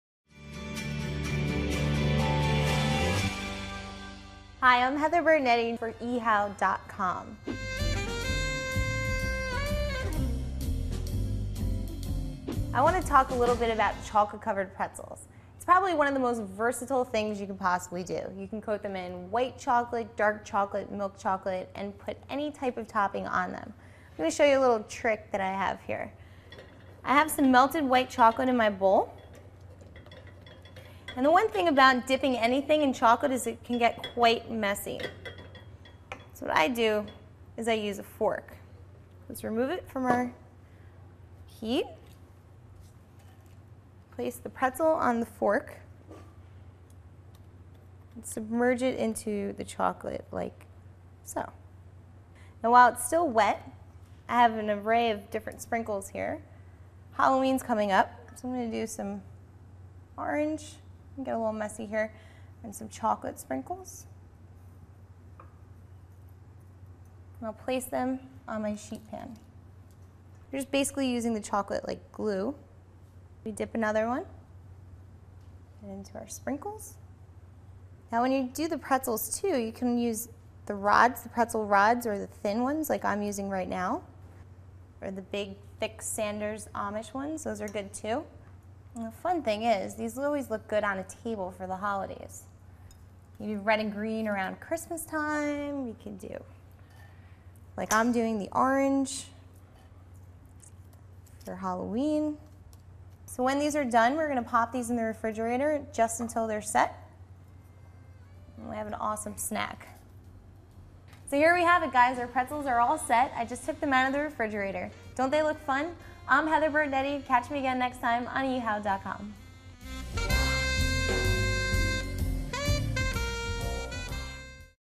Sweet & Salty Chocolate-Covered Pretzels
Sweet & Salty Chocolate-Covered Pretzels. Part of the series: Piece of Cake. Equally good for snacking and gifting, pastry chef Heather Bertinetti’s sweet and salty chocolate pretzels can be assembled in no time.